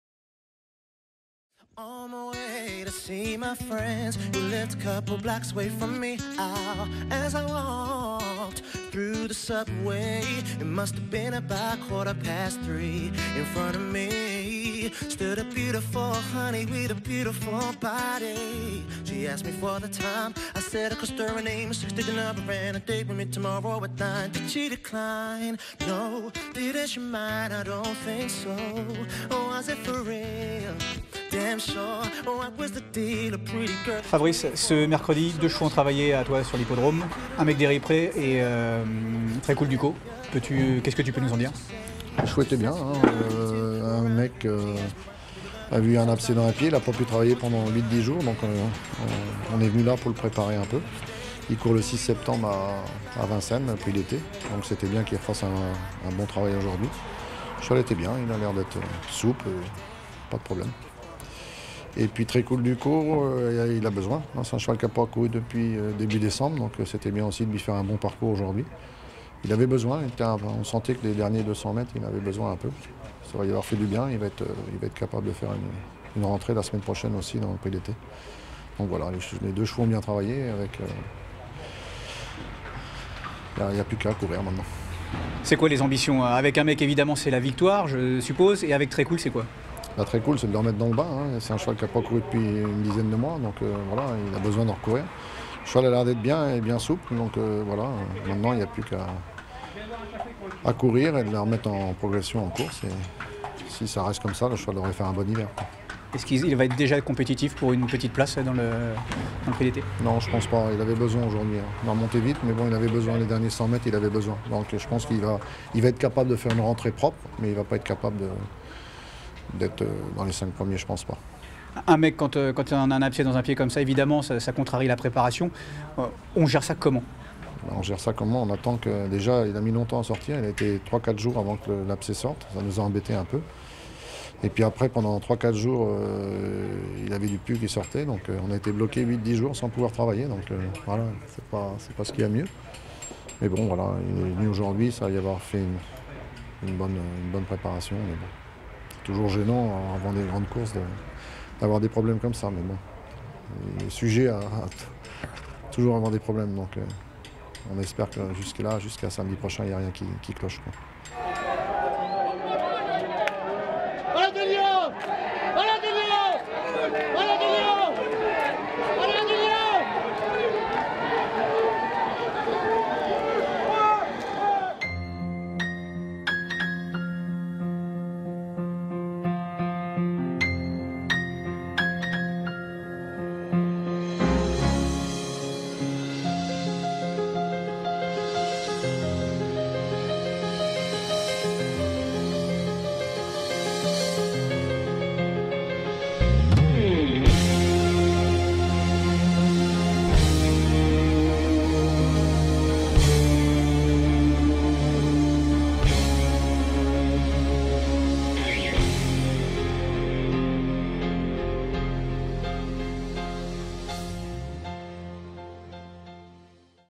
TRESKOOL DU CAUX et UN MEC D'HERIPRE à Vincennes ce mercredi - A2TURF.COM
Deux des très bons chevaux de Fabrice SOULOY étaient en piste avant la première épreuve ce mercredi à Vincennes : TRESKOOL DU CAUX et UN MEC D'HERIPRE.
Les caméras d'a2turf étaient là...